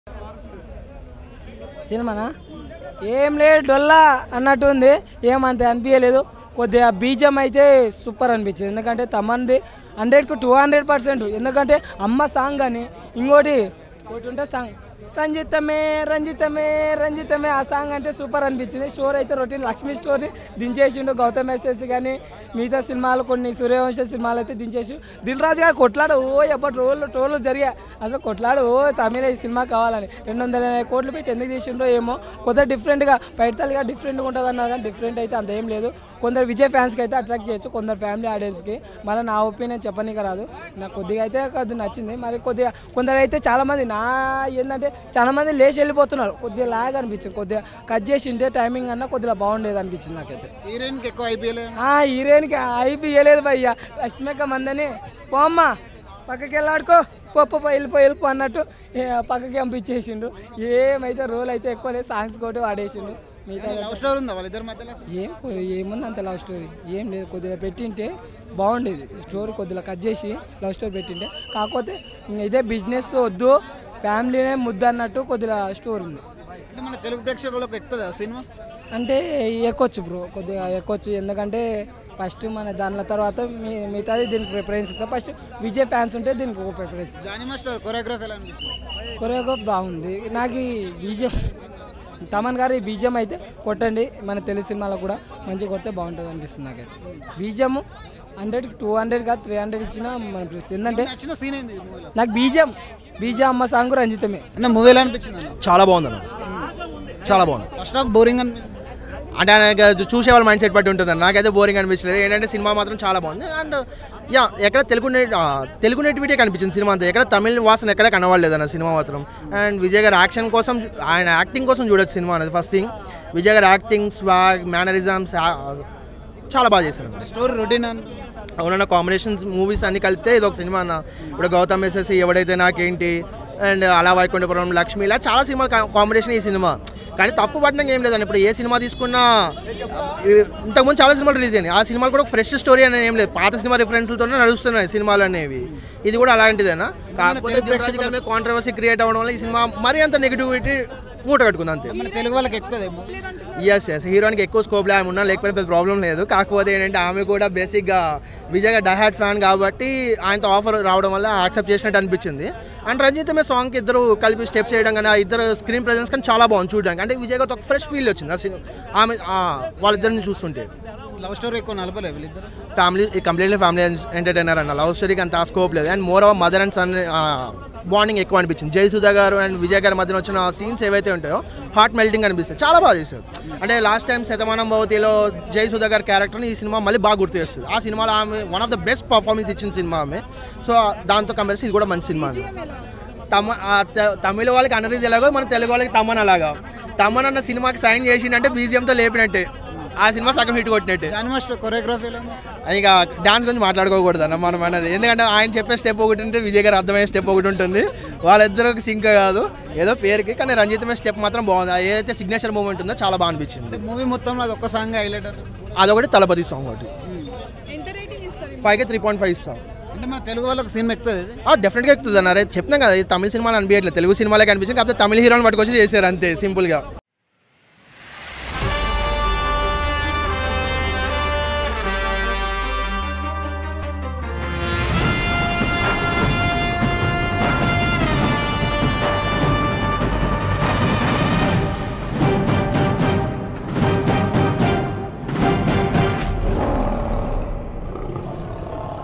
Vaarasudu Movie Genuine Public Talk | Thalapathy Vijay | Rashmika | Vaarasudu Movie Review | Rating
Leo Politics

Leo Health

Leo Entertainment

Leo Devotional